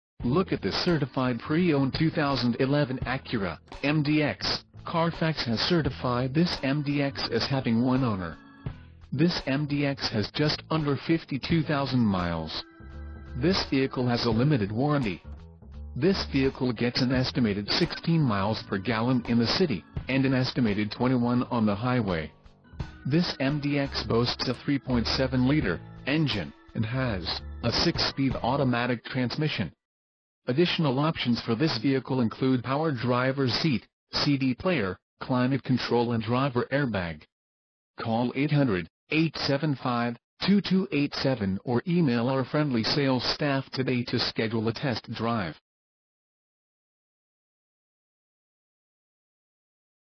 2011 Acura MDX Technology Entertainment Package SUV - Overland Park, KS
Hendrick Acura Overland Park, Overland Park, KS - Navigation, Backup Camera, XM Radio and SH AWD